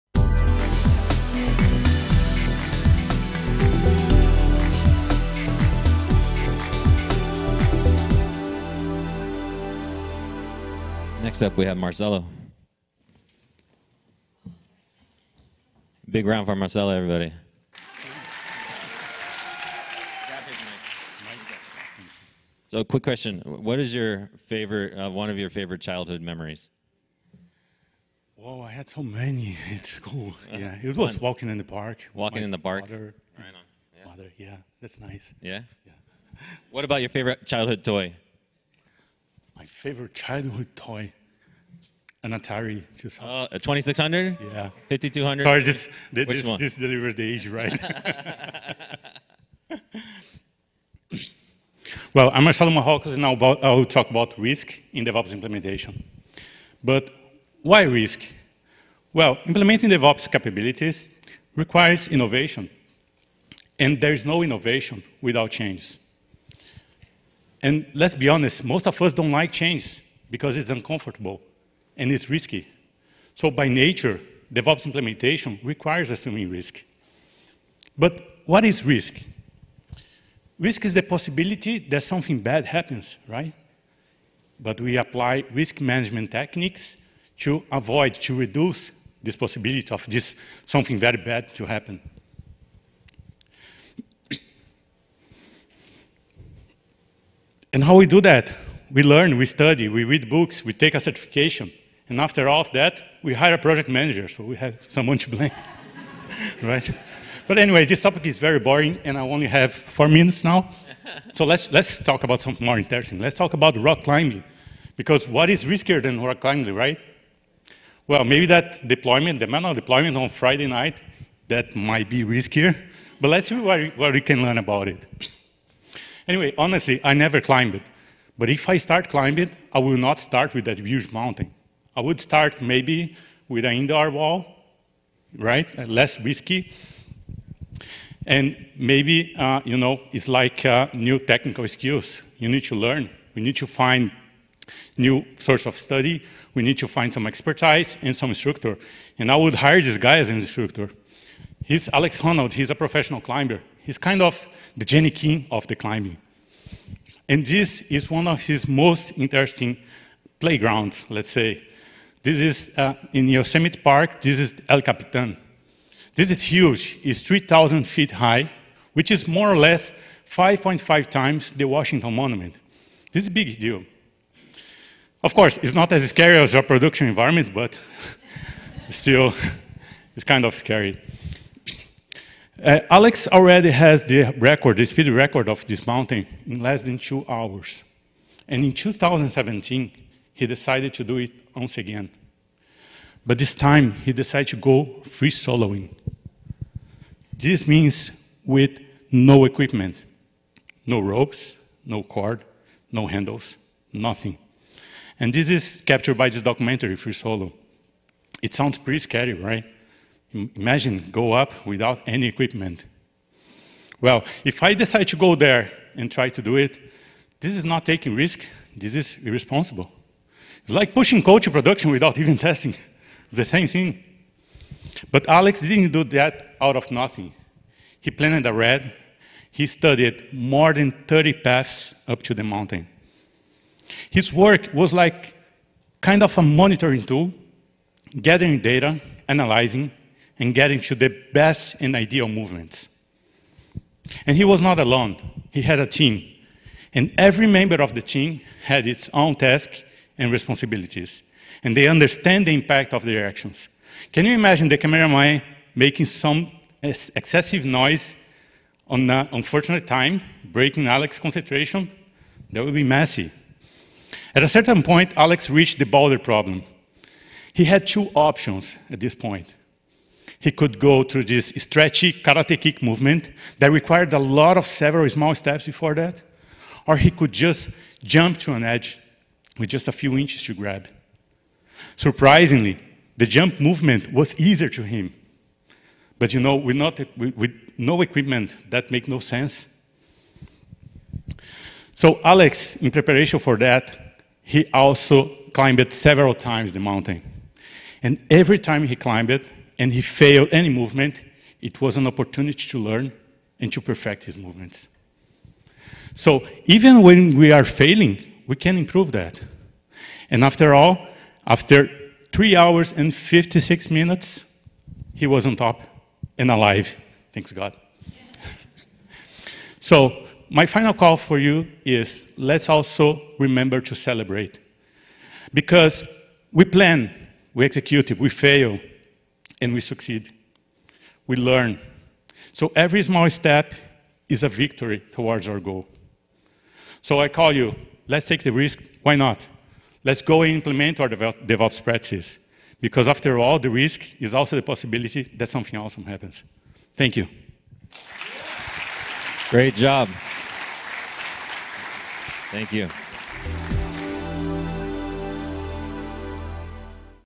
Marcello Marrocos - Risk: The possibility that something happens
DevOps transformation requires innovation. Innovation leads to changes. Changes require risk. So, in a DevOps transformation, taking risks is often necessary. Let's explore some aspects of risk and risk management, such as expertise, challenges, failure anticipation, perspective, and the difference between risk and irresponsibility from a different and non-technical point of view.